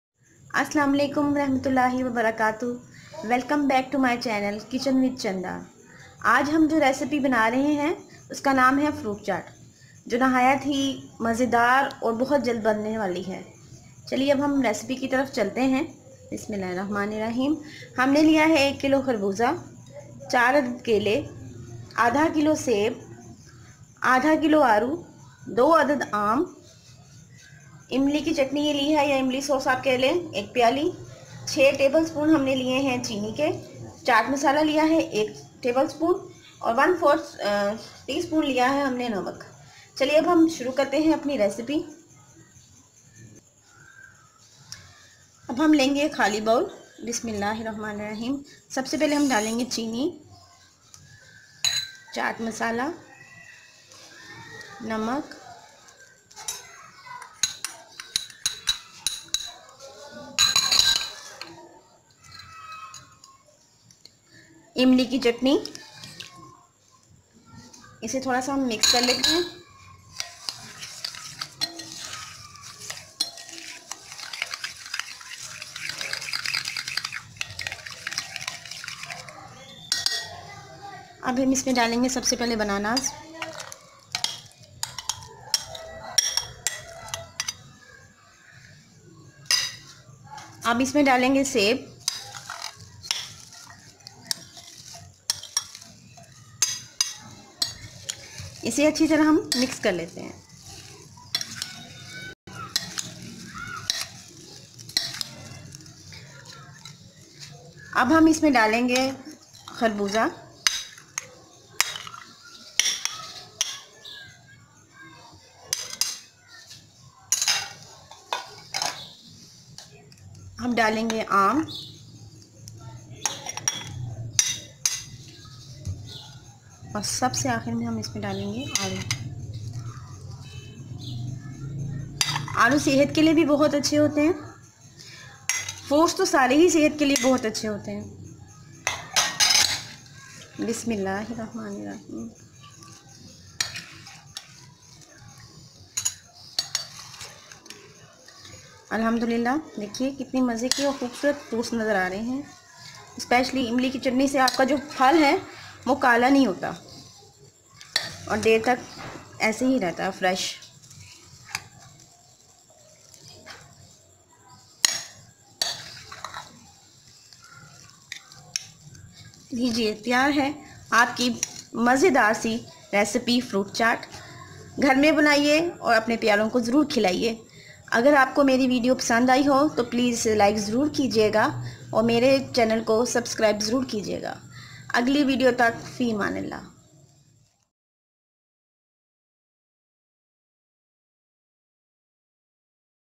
Fruit Chat Recipe| Delicious Fruit Chat Recipe
instant fruit chat

This Video is about Homemade fruitchat